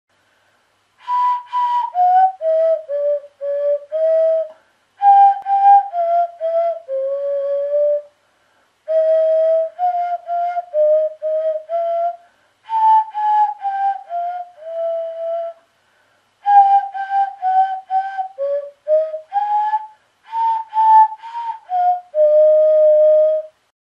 スワラヒドゥン -インドネシア楽器-
口を開けて上側のくぼみに鼻を押しあてて鼻息を吹くと、息が口の中に共鳴して音が出ます。